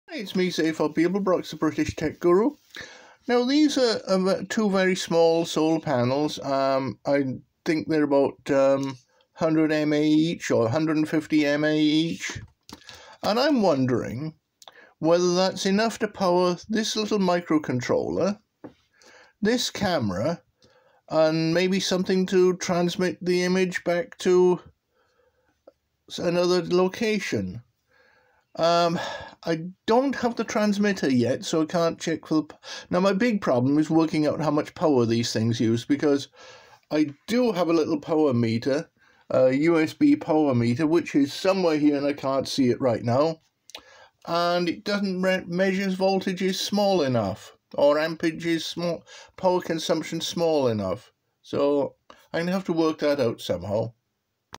Remote camera potential
All this needs is a transmitter to transmit images captured and this could be a very long range remote image sending gizmo.